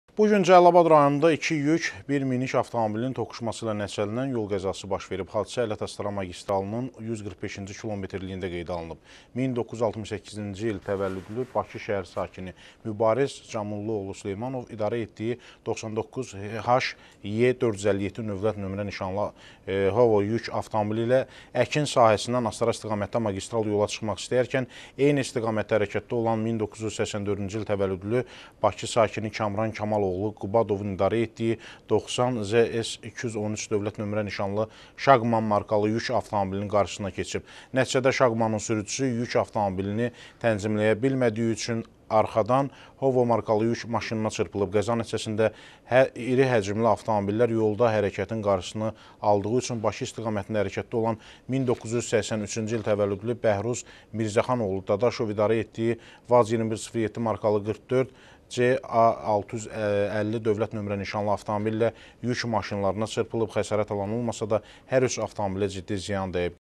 Cəlilabadda 3 avtomobil toqquşdu, xəsarət alan olmadı
Cəlilabad rayonunda iki yük, bir minik avtomobilinin toqquşması ilə nəticələnən yol qəzası baş verib. Hadisə Ələt-Astara magistralının 145-ci kilometrliyində qeydə alınıb.1968 -ci il təvəllüdlü Bakı şəhər sakini Süleymanov Mübariz Camulla oğlu idarə etdiyi “99 HE 457” dövlət nömrə nişanlı “HOWO” markalı yük avtomobili ilə əkin sahəsindən Astara istiqamətinə magistral yola çıxmaq istəyərkən eyni istiqamətdə hərəkətdə olan 1984-cü il təvəllüdlü Bakı sakini Qubadov Kamran Kamal oğlunun idarə etdiyi “90 ZS 213” dövlət nömrə nişanlı Şaqman markalı yük avtomobilinin qarşısına keçib. Nəticədə Şaqmanın sürücüsü yük avtomobilini tənzimliyə bilmədiyi üçün arxadan “HOWO” markalı yük maşınına çırpılıb. Qəza nəticəsində irihəcmli avtomobillər yolda hərəkətin qarşısını aldığı üçün Bakı istiqamətində hərəkətdə olan 1983-cü il təvəllüdlü Dadaşov Bəhruz Mirzəxan oğlu idarə etdiyi “VAZ - 21 07” markalı “44 CA 650” dövlət nömrə nişanlı avtomabillə yük maşınlarına çırpılıb. Xəsarət alan olmasa da, hər üç avtomobilə ciddi ziyan dəyib. Qeyd edək ki, qəza vaxtı hər iki yük avtomobili yüklü vəziyyətdə olub. Hazırda faktla bağlı araşdırma aparılır.